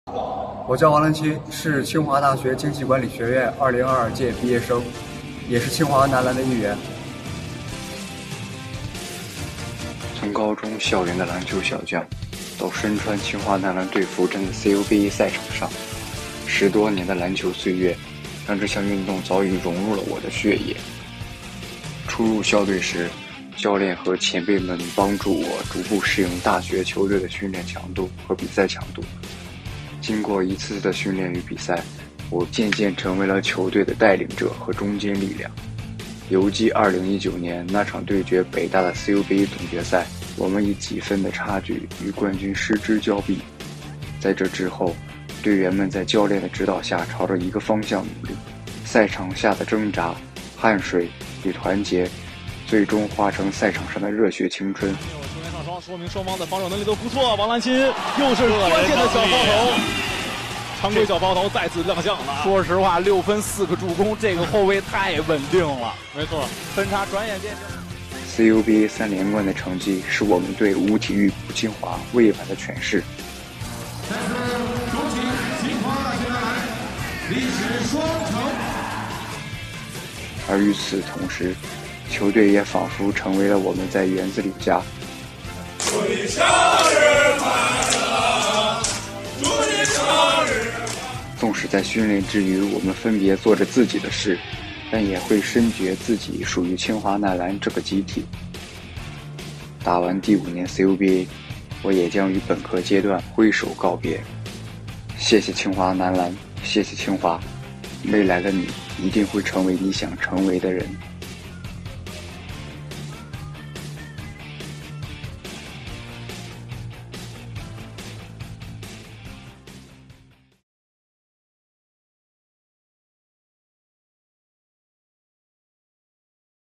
Graduation story of 2022 graduate Wang Lanqin
Wang Lanqin, a graduate of the School of Economics and Management from the Class of 2022, was a member of the Tsinghua University Men’s Basketball Team. As a leader of the team, he led the team to achieve the goal of three consecutive CUBA championships. Check the video out to know more about his story at Tsinghua University!